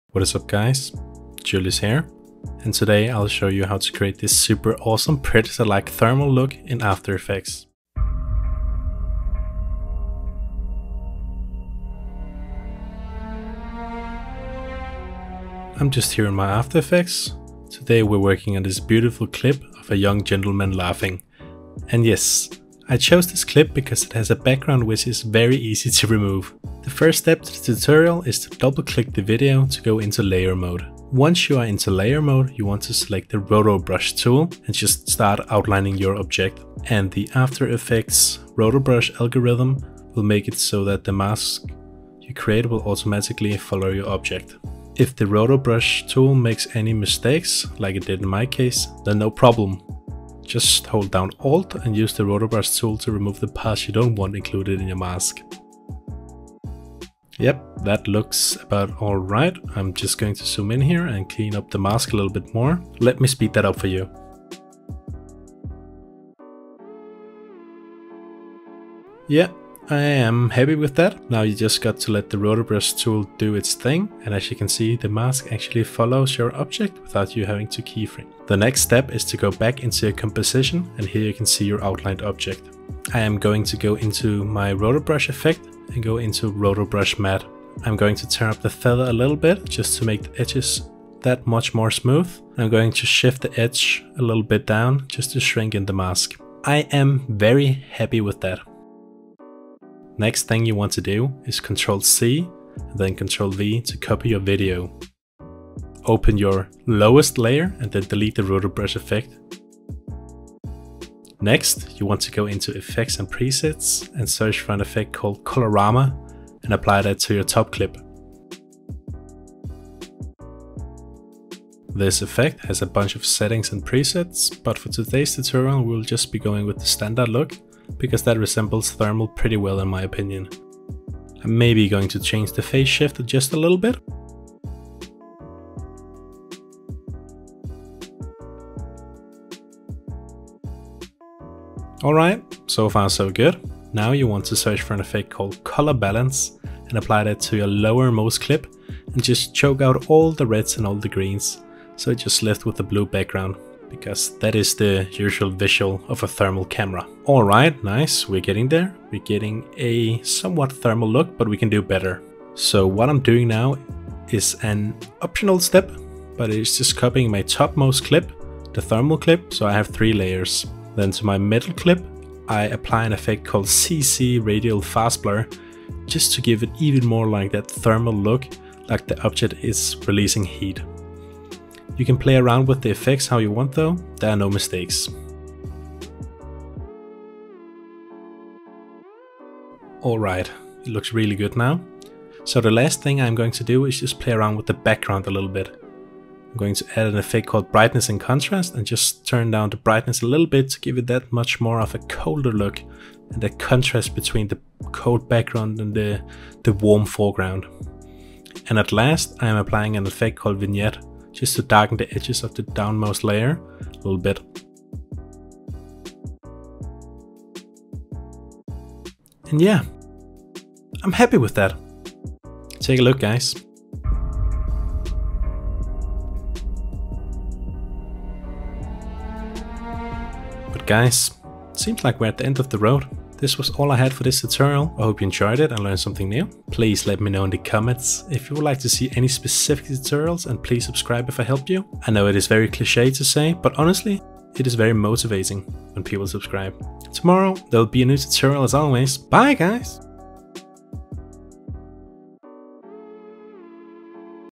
Thermal Vision Look in After Effects (Heat Mapping Effect)
✗ This video is a tutorial on how to get a Thermal Vision look and a heat mapping effect in After Effects

In this tutorial I will go over how to create a thermal vision look and a heat mapping effect in adobe after effects 2022. I will teach you how to use the roto brush tool, how to use the colorama effect, how to use the color balance effect, how to use the brightness and contrast effects as well as how to use the vignette effect. It does not require anything else to get such a super sleak and cool look! Super simple!